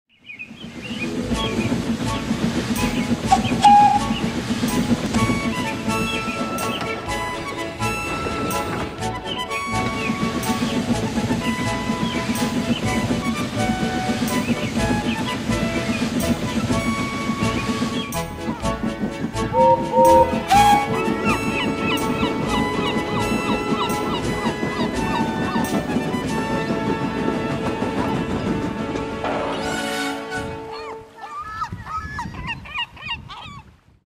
Jinty's Harbor Delivery
Jinty just making a harbor delivery.

SFX & Music by carson08022000, G.W. Shunter, SoundJay,  Brandon Polley, Rhydyronen, TheGermanOfSodor, AceOfTrains, Bladez, Yoshihisa Hirano, Michael Melton, Mavis M, and Sudrian Afro.